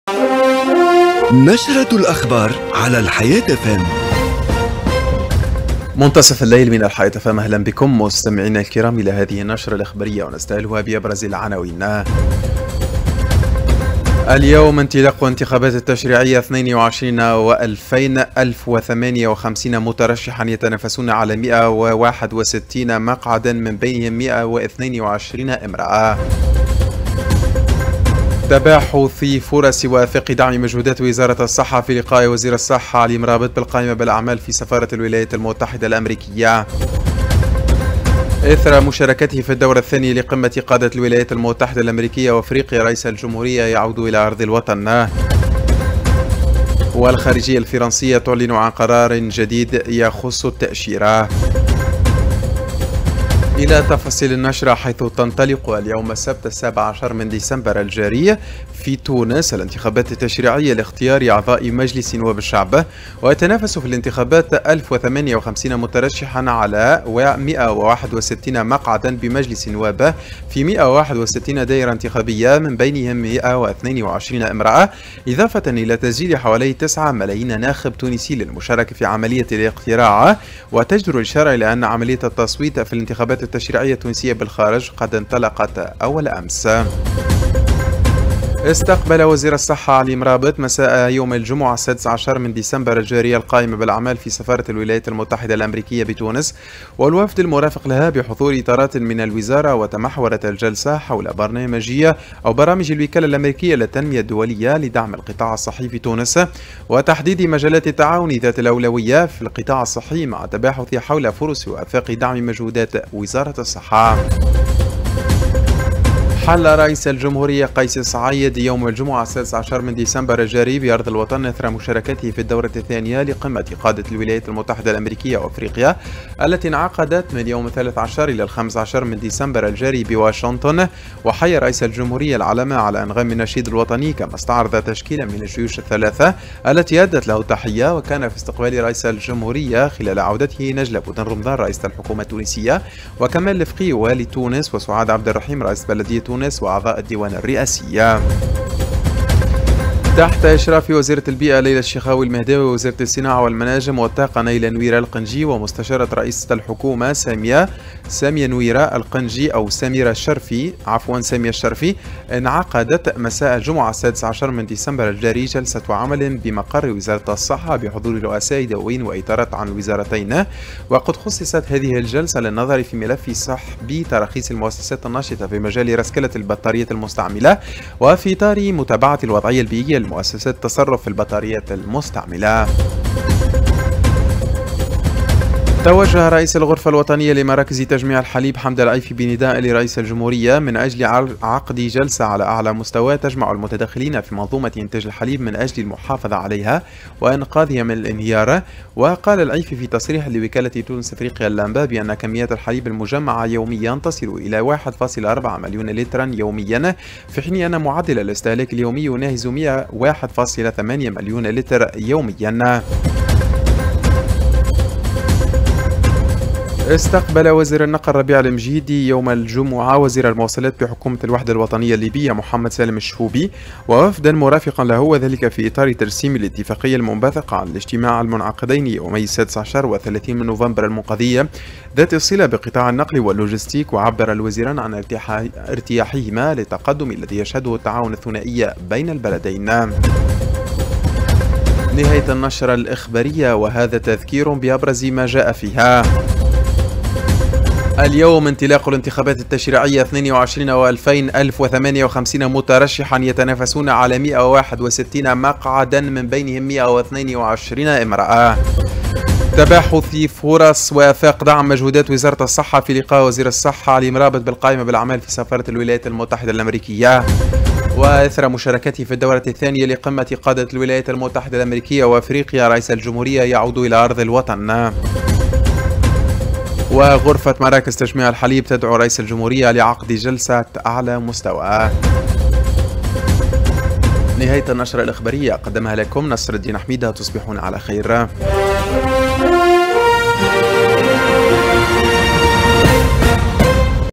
نشرة الاخبار: اليوم انطلاق الانتخابات التشريعية 2022
اليوم  انطلاق الانتخابات التشريعية 2022 : ألف و58 مترشحا يتنافسون على 161 مقعدا ، من بينهم 122 امرأة
البث المباشر🎙📻🎚